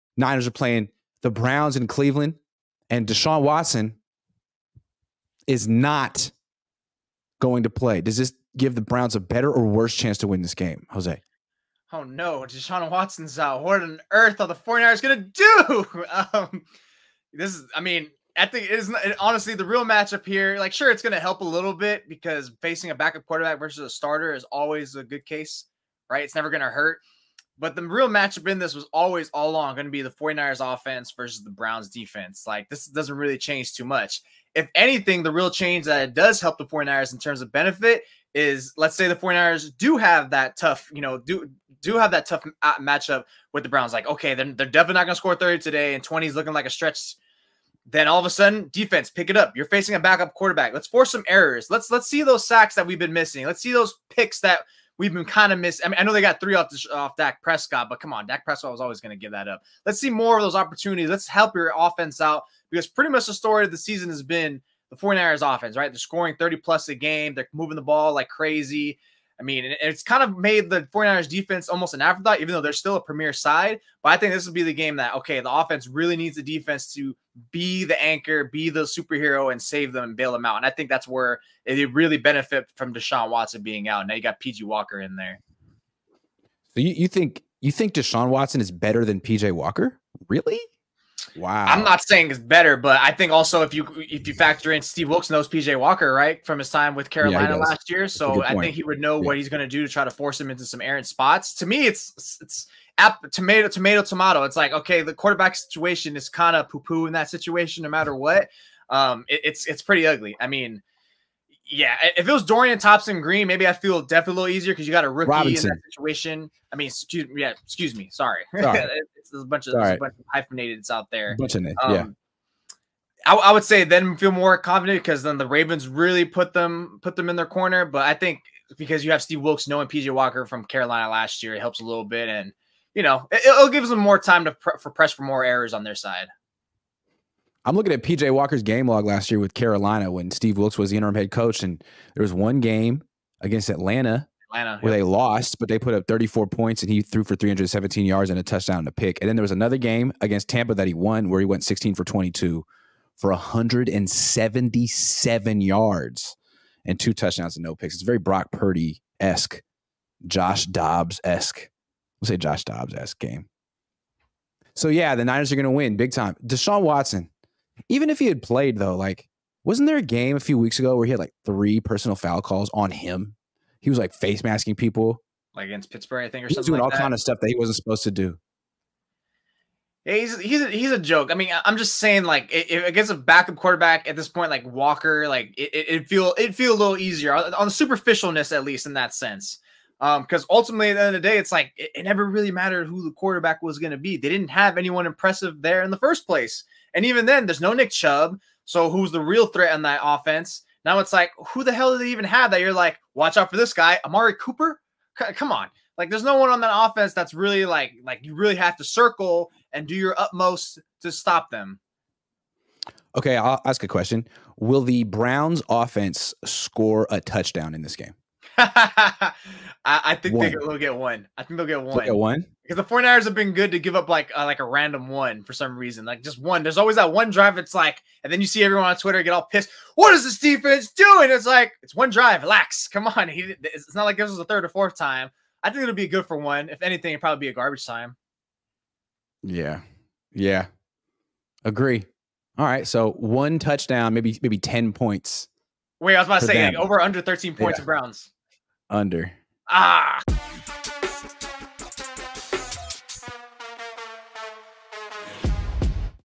Browns QB Deshaun Watson will be OUT Against the 49ers
Grant Cohn and Jose Sanchez discuss the news that Cleveland Browns quarterback Deshaun Watson will be out against the San Francisco 49ers.